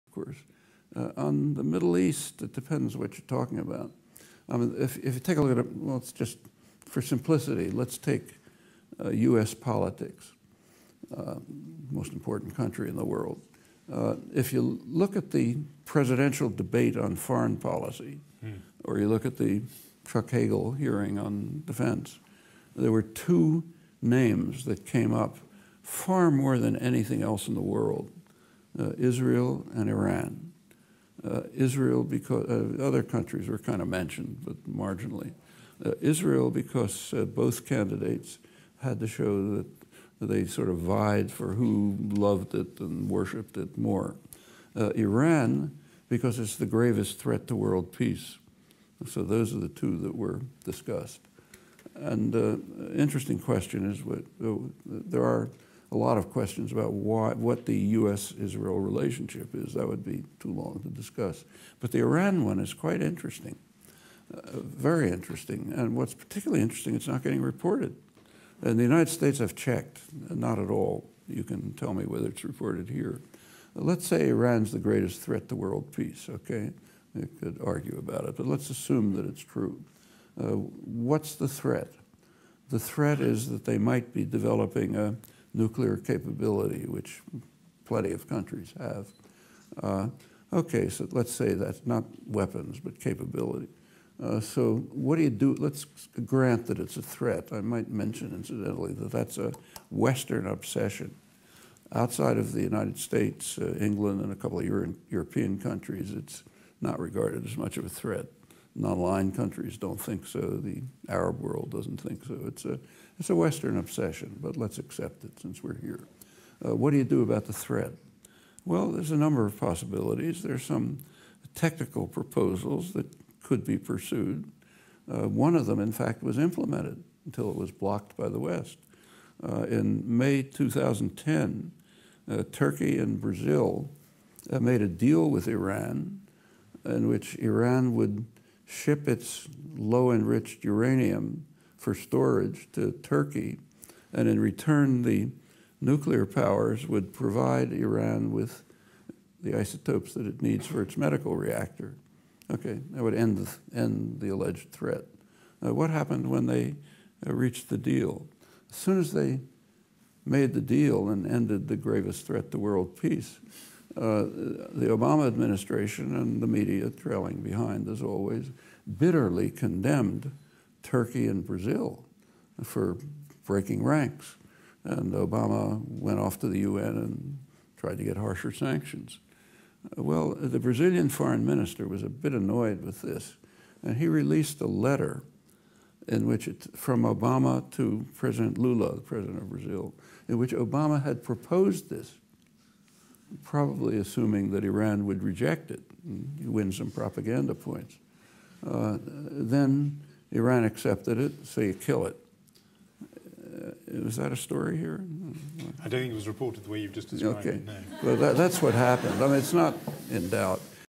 Chomsky on Iran Nuclear Deal w Turkey Brazil and the US propaganda around it [Farsi Subs]
Noam Chomsky tells the story of a deal between Iran on one hand, and Turkey and Brazil on the other hand that could have ended Iran's nuclear crisis, and how the US thwarted it for the good of its own propaganda.

The link to full interview is /watch?v=O0D0E42AA4I